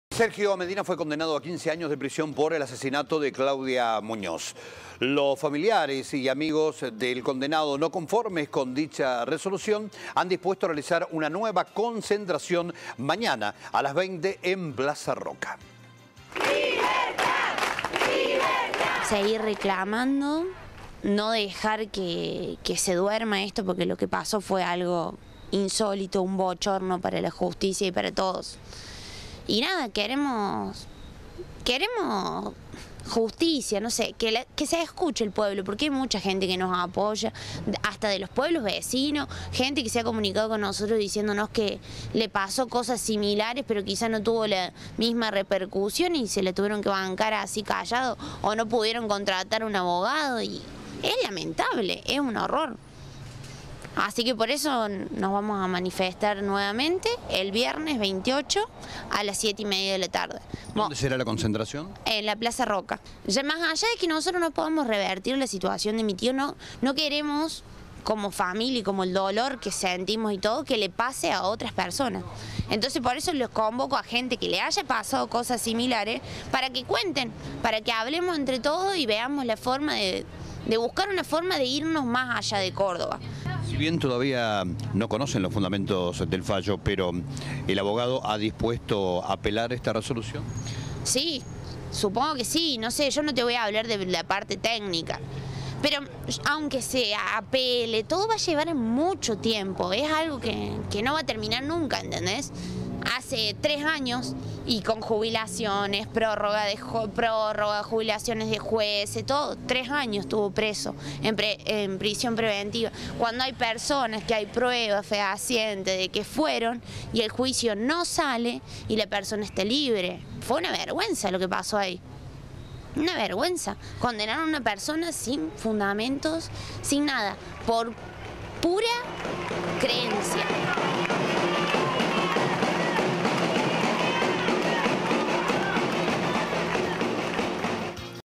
La familia de Sergio Medina convoca a una nueva marcha para reclamar su libertad
Este viernes a las 20hs, convocan a una concentración en Plaza Roca para reclamar justicia y libertad por Sergio Medina, quien fue condenado a 15 años de prisión por el crimen de Claudia Muñóz.
Anabella Reineri, sobrina de Medina, expresa que otro de los objetivos de la marcha será "dar a conocer otros casos similares, donde se condenó injustamente" e "impedir que vuelva a ocurrirle a otra persona", refiriéndose a que su tío fue condenado sin pruebas concretas.